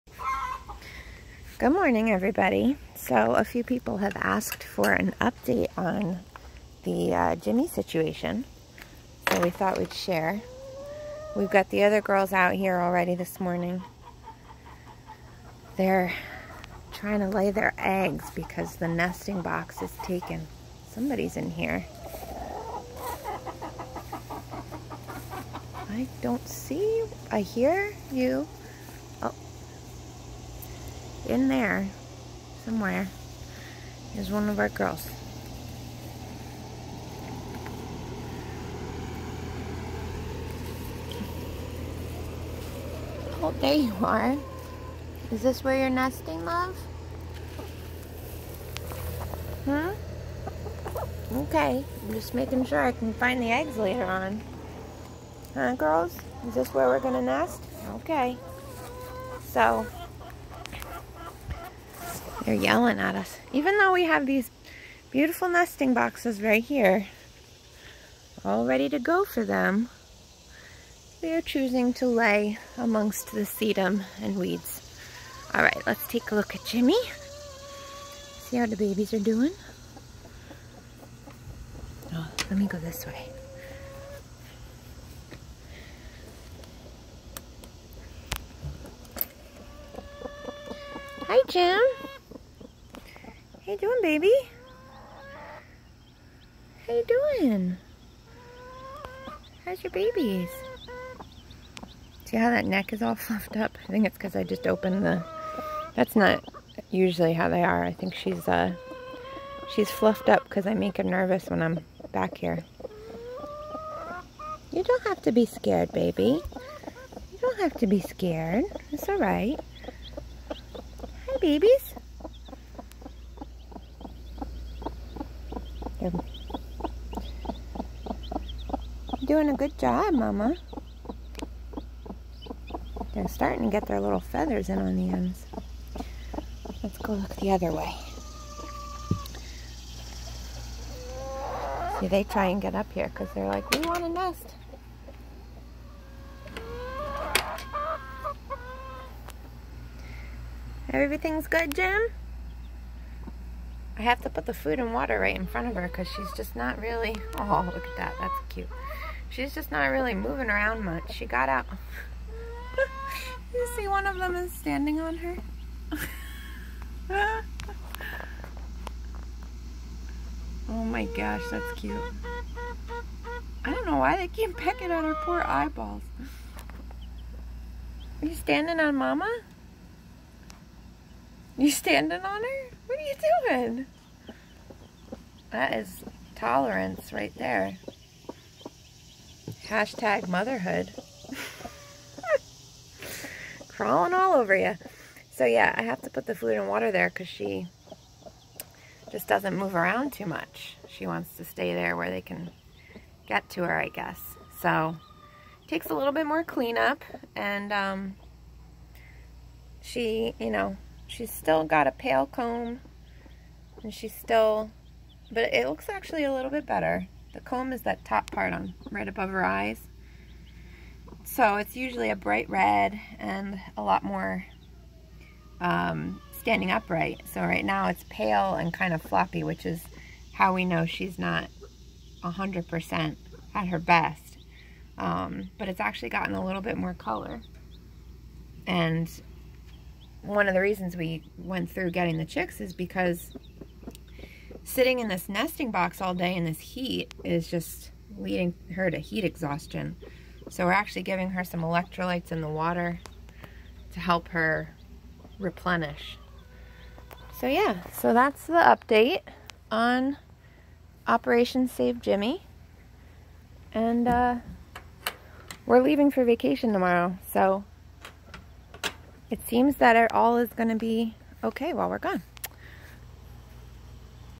SaveJimmy the broody mama hen, day 2
Update on on out broody hen, Jimmy, and her foster chicks!